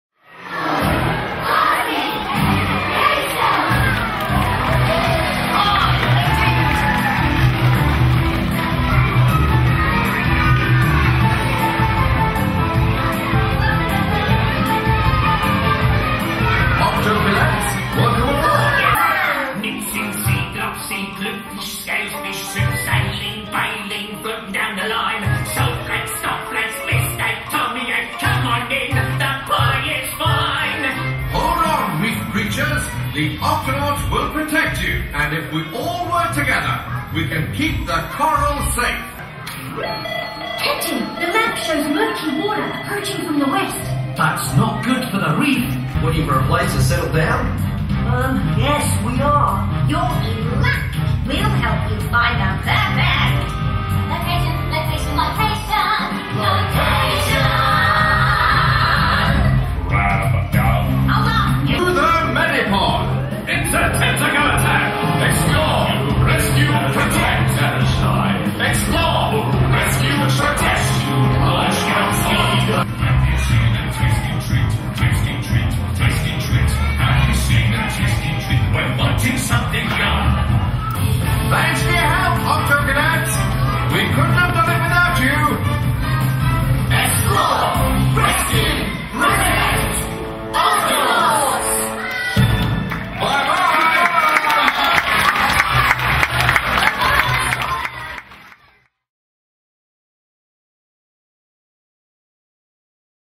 Octonauts Live! Operation Reef Shield (Australian Tour) - Official
Captain Barnacles and his trusty crew join forces with a host of colourful Australian reef dwellers on a mission to save the Great Barrier Reef! When the Octonauts deploy a new undersea medical station designed to keep the reef clean and healthy, they discover a problem that threatens the reef dwellers and their home. An outbreak of spiky and very hungry Crown of Thorns Starfish are on their way to eat the reef! It will take all the Octonauts, their Gups, and a rag-tag crew of sea creatures to ward off the advancing starfish and save the Great Barrier Reef!